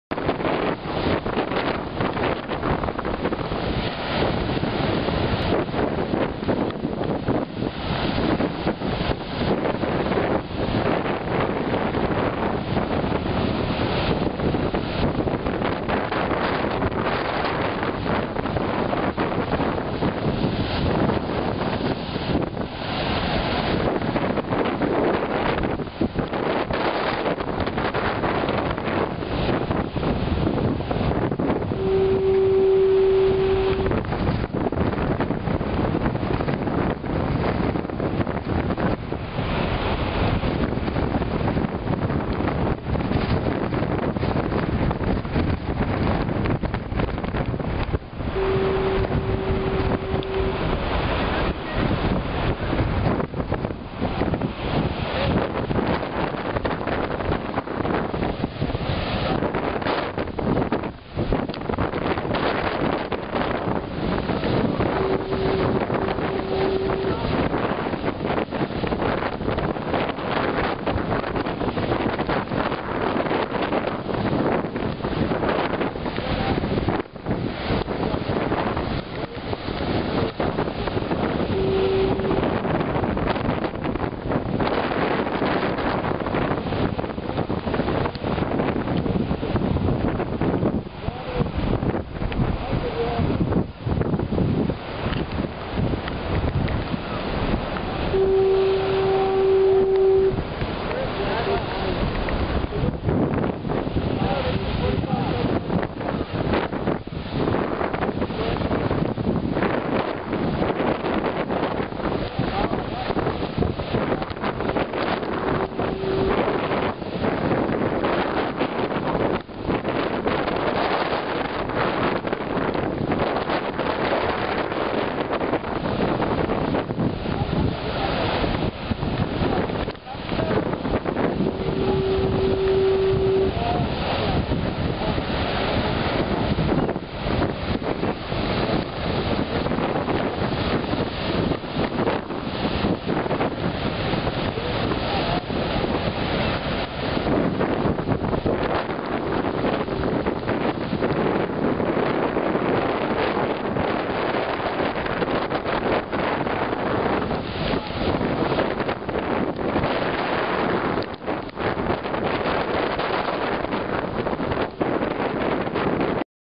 Post Nor'easter waves 4/17/2007 - 7 of 7
Post-Nor'easter waves 4/17/2007 at Fort Williams in Cape Elizabeth Maine